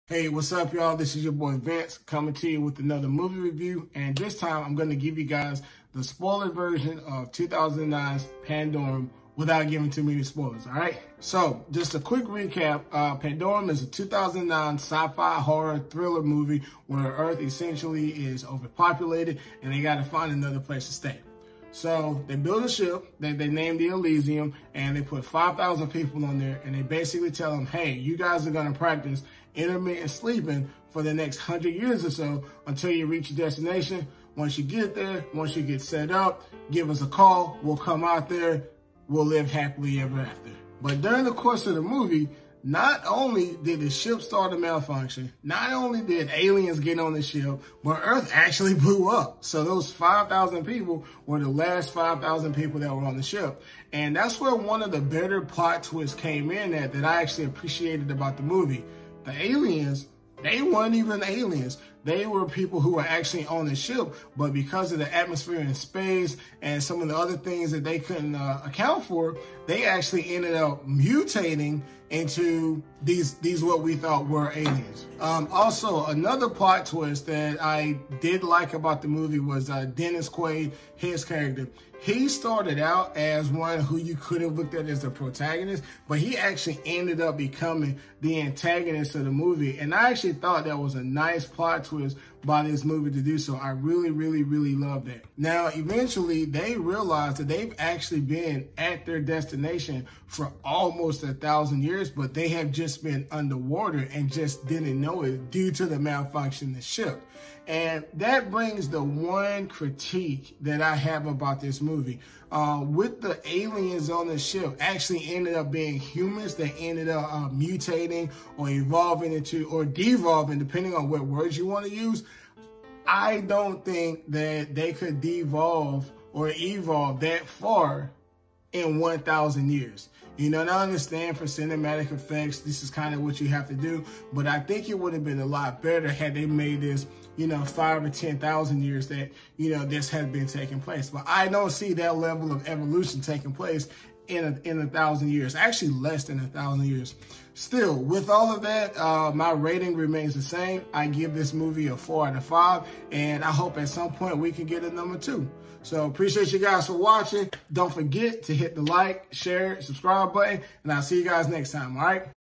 Quaranview 3: Pandorum *SPOILERS*
Giving my review of Pandorum this time with spoilers.